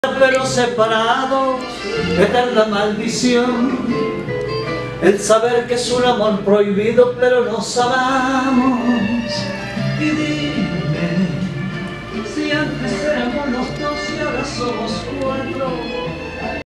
GALY GALIANO CANTA EN NUESTRA CABINA
Siempre los mejores delante de nuestro micrófono, Galy canta y encanta con todos sus 30 años de carrera.